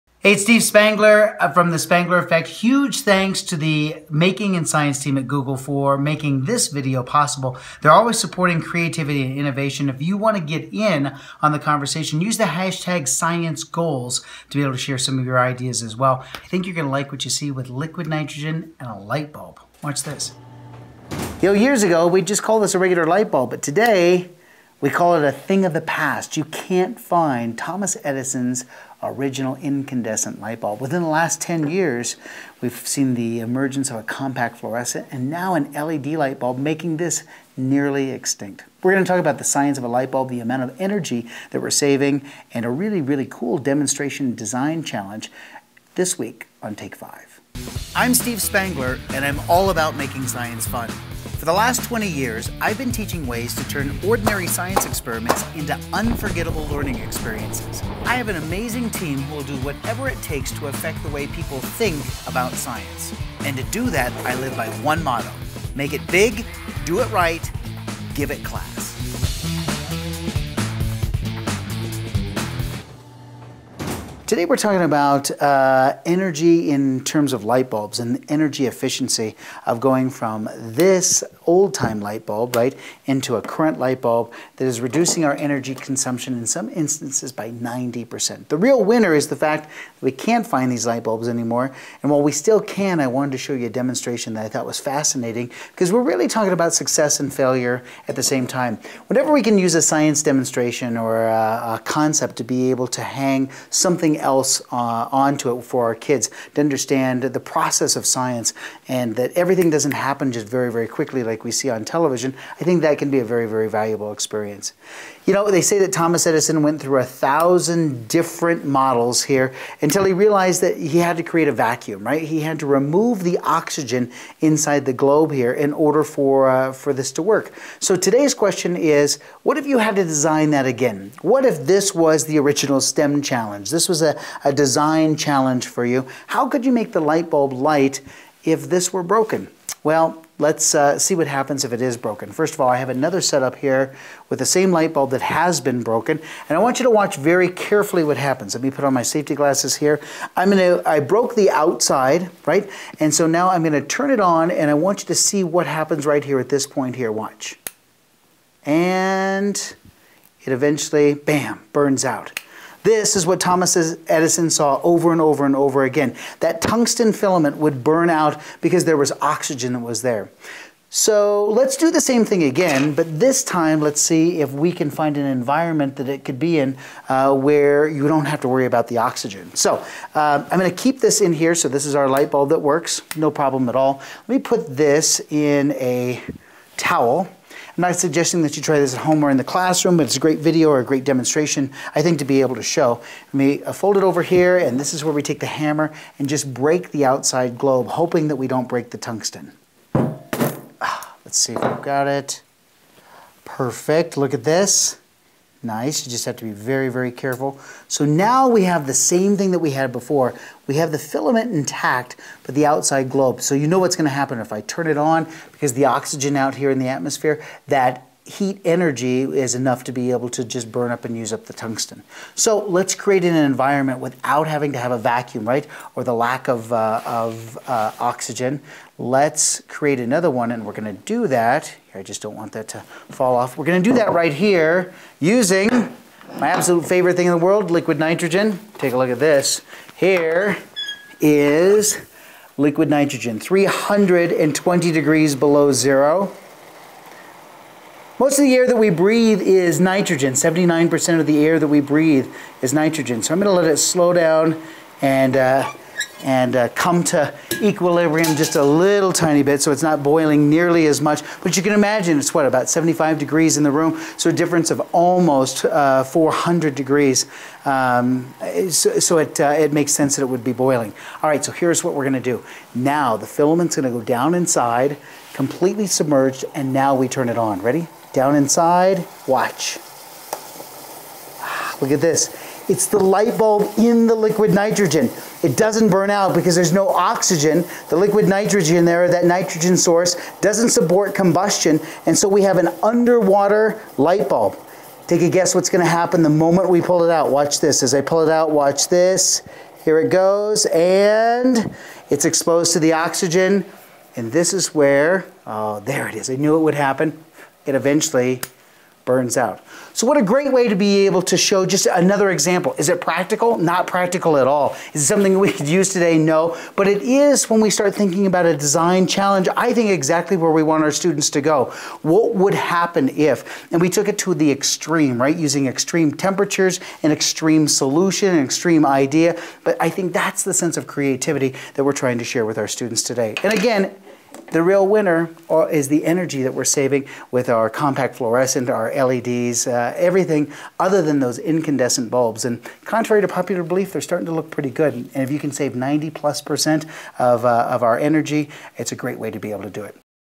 Light bulb in Liquid Nitrogen - The Spangler Effect
Thanks to the Making & Science team at Google for their support in making this video possible!

Join the conversation and share your ideas with sciencegoals

Steve Spangler demonstrates why the filament in a light bulb is incased in the glass bulb, and shows how to keep the bulb lighted without the glass.

Join Steve for one of his hands-on science workshops.